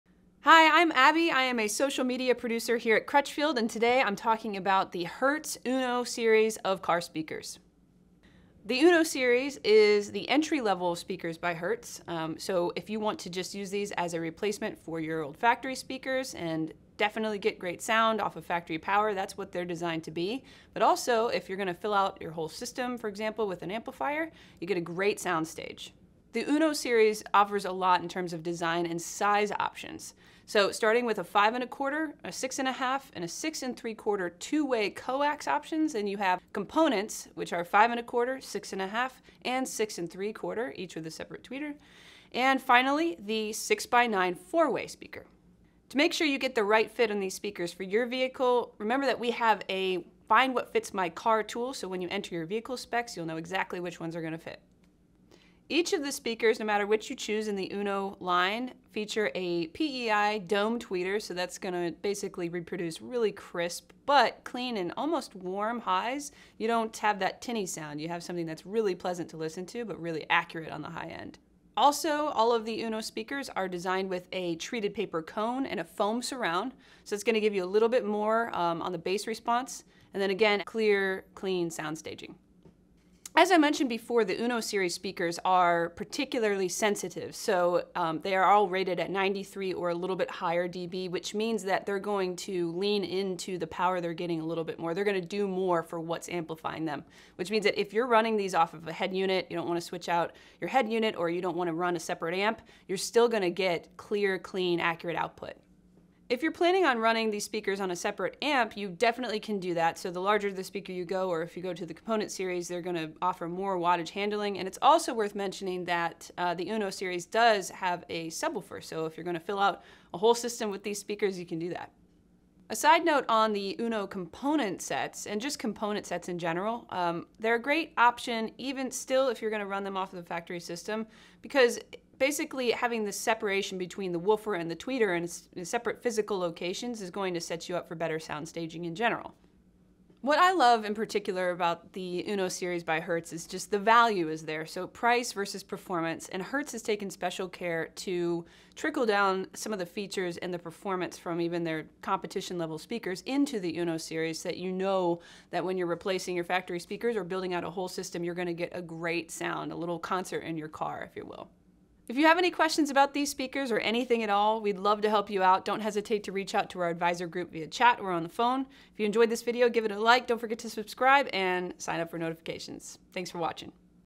Hertz Uno series car speakers | Crutchfield
Hertz Uno series speakers are built to deliver great sound even when powered by your factory stereo, making them great drop-in replacements if they fit in your car. They can also handle a decent amount of power if you're adding amplifiers, and most importantly they sound incredible in your car.

Hertz Uno series speakers are very sensitive, so they don't need a lot of power to deliver great sound. Even underpowered factory systems can make them sing, so if you want better sound in your car but don't want to go crazy with your project, a set of Hertz Uno speakers might just be a perfect choice.

0:00 Intro
0:26 Available in many sizes and configurations
0:55 Built better than your factory speakers
1:23 High sensitivity
1:46 Good power handling
2:02 Component sets
2:20 Great price-to-performance value
2:42 Recap and contact info